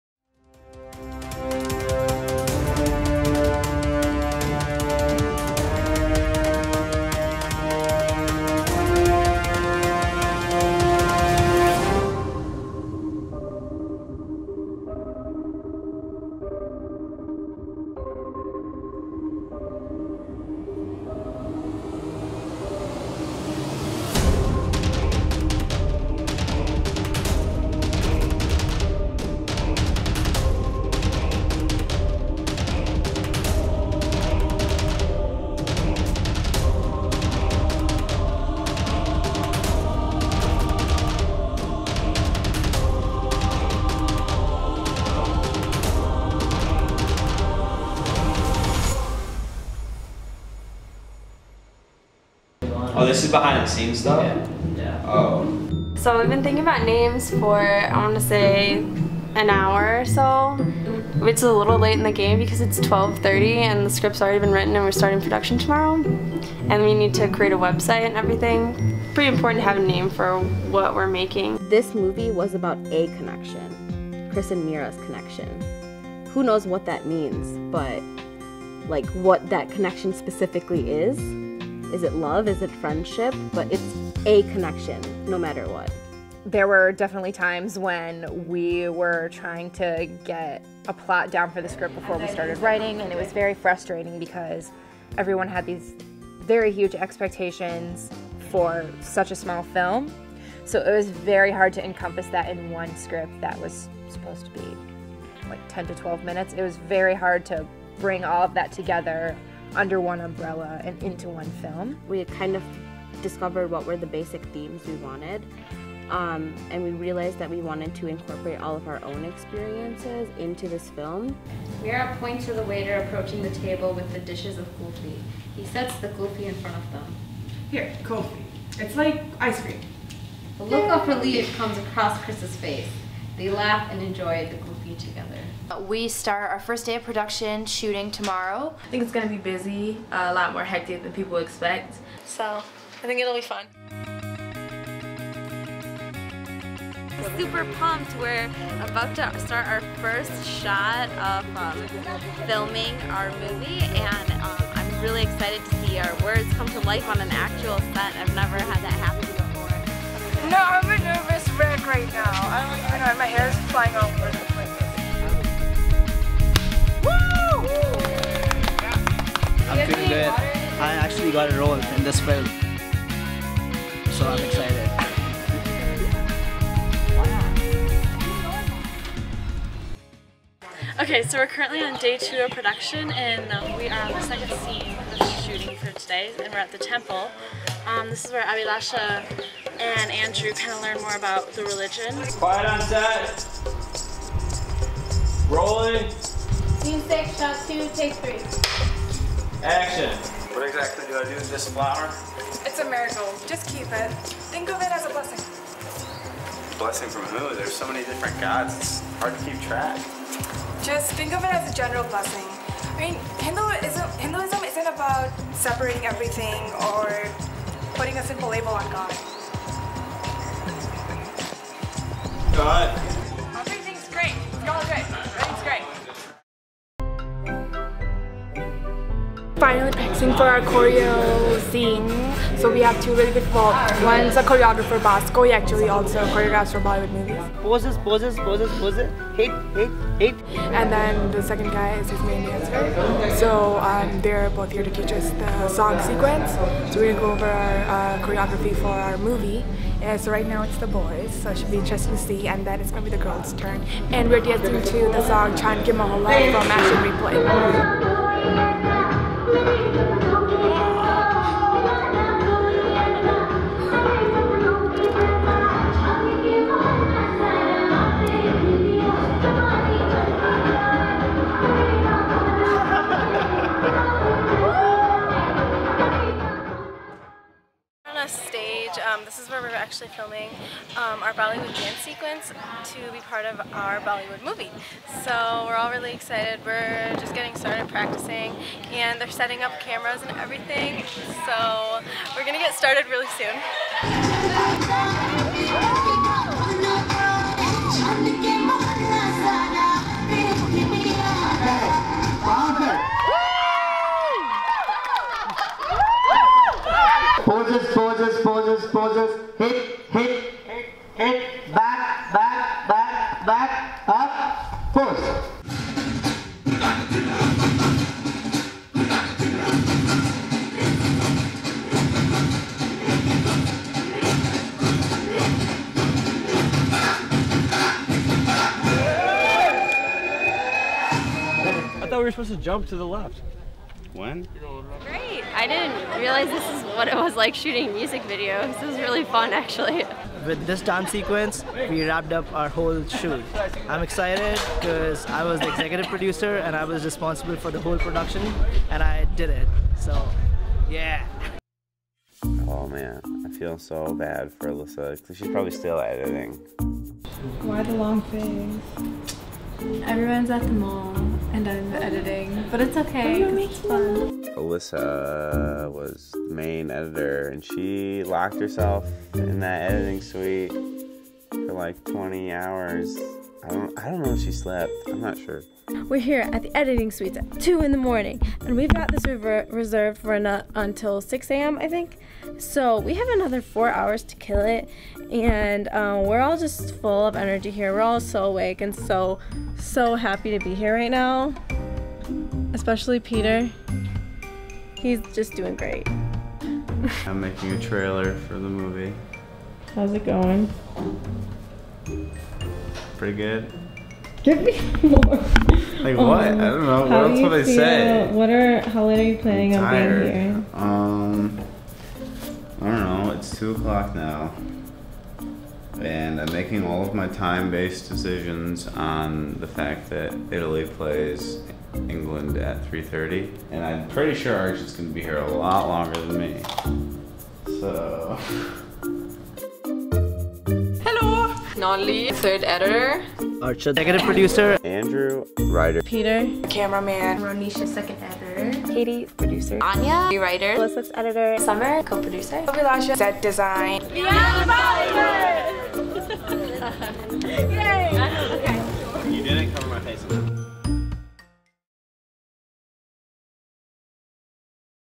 Making of "A Connection"
Making of a short film "A Connection". Shot and edited by Archit Batra and Katie Zurburg